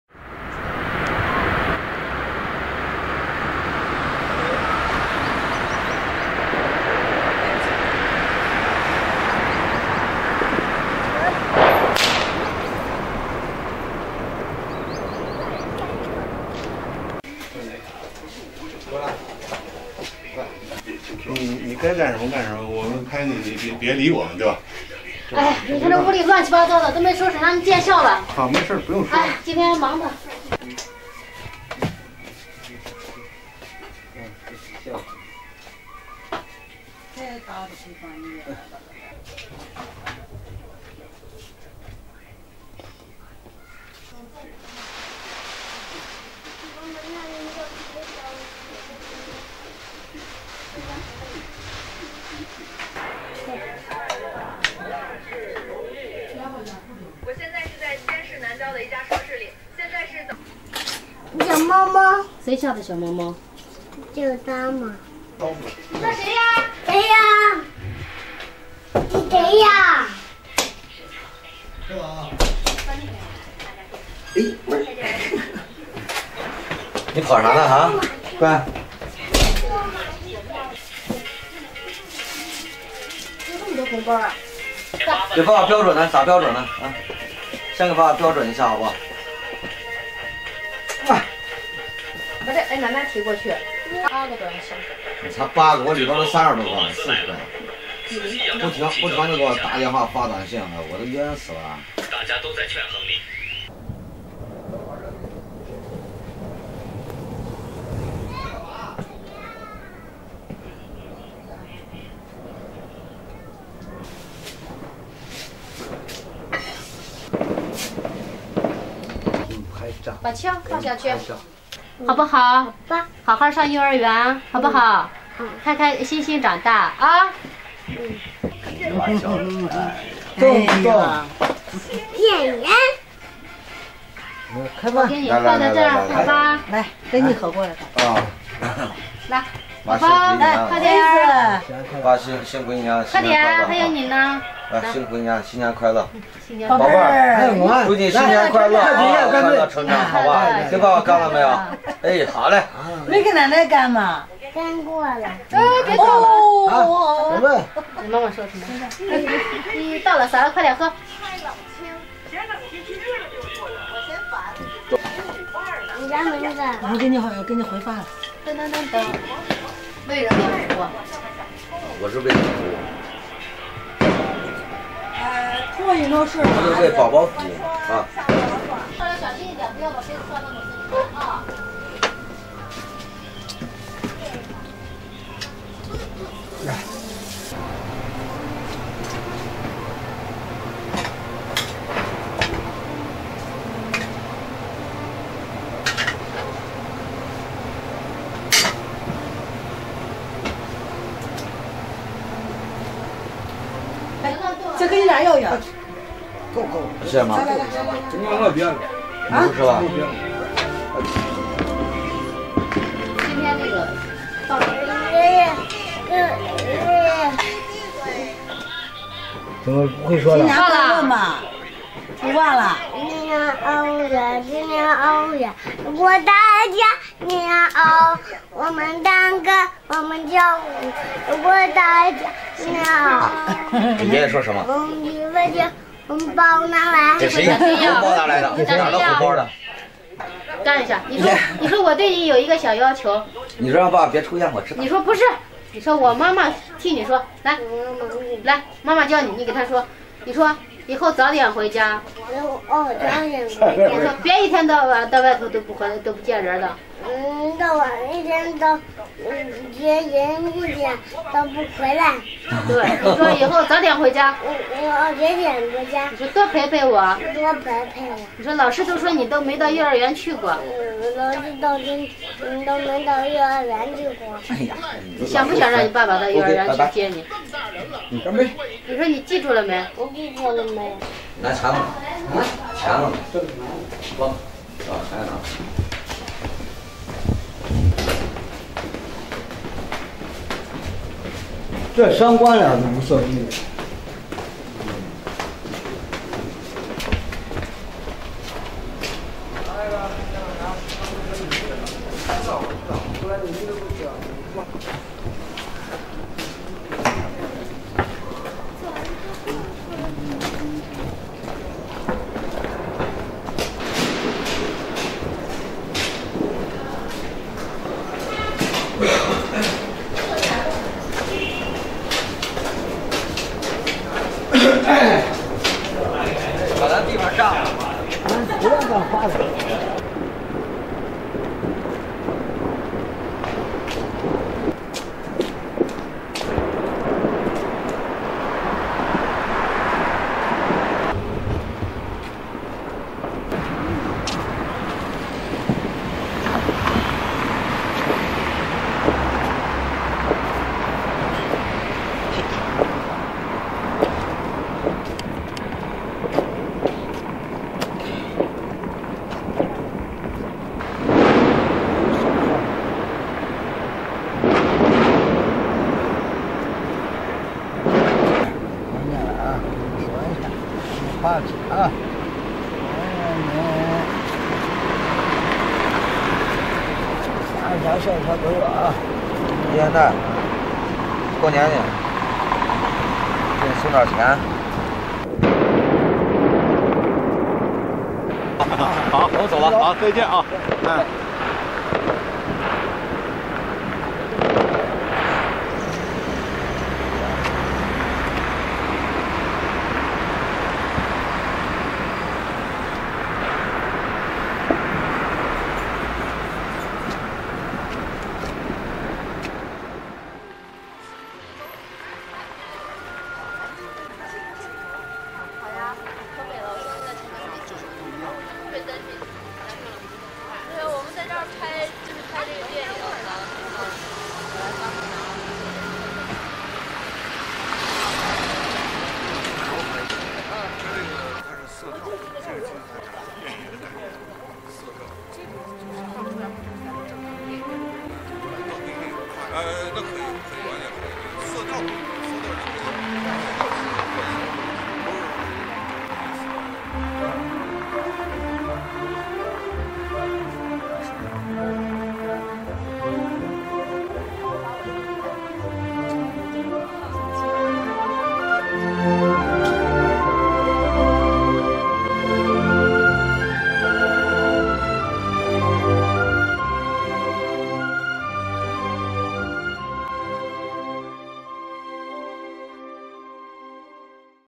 寻味 年
本片记录了一个极其普通人家的除夕之夜